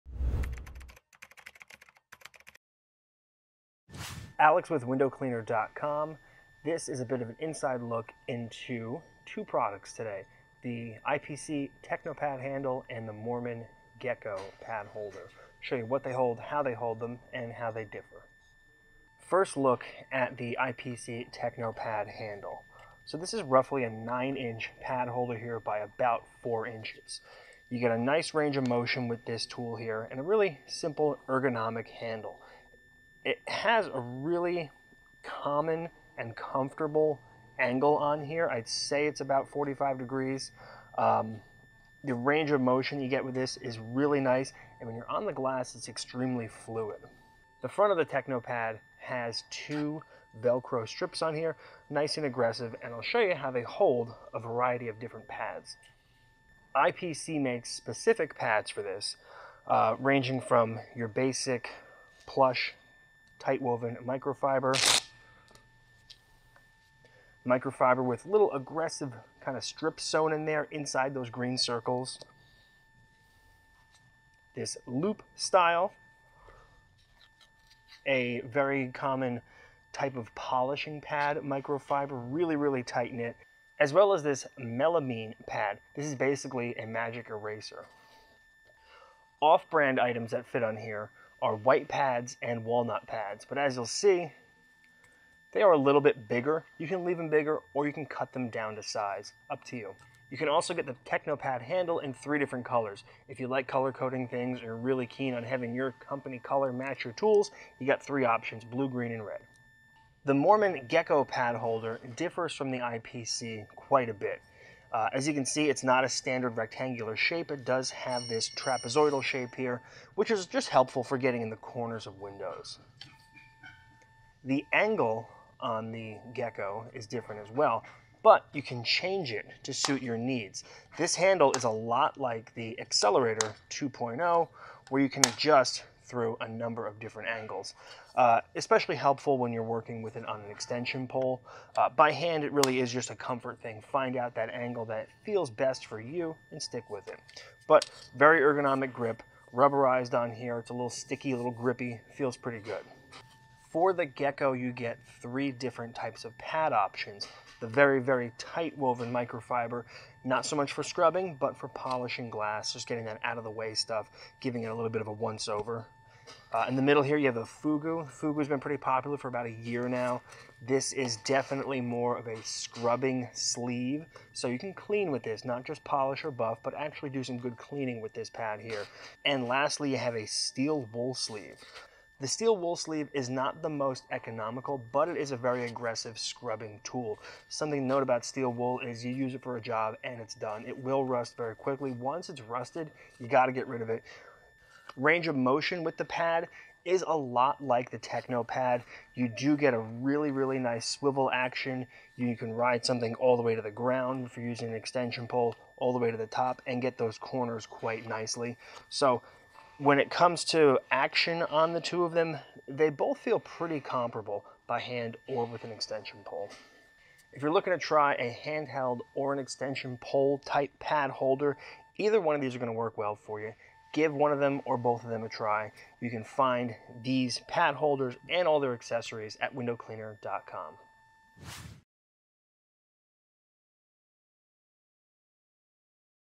Moerman Gekko VS IPC Technopad
Having a hard time deciding what pad holder is right for you? Well, Alex is here to help by comparing and contrasting the new Moerman Gekko and the IPC Eagle Technopad!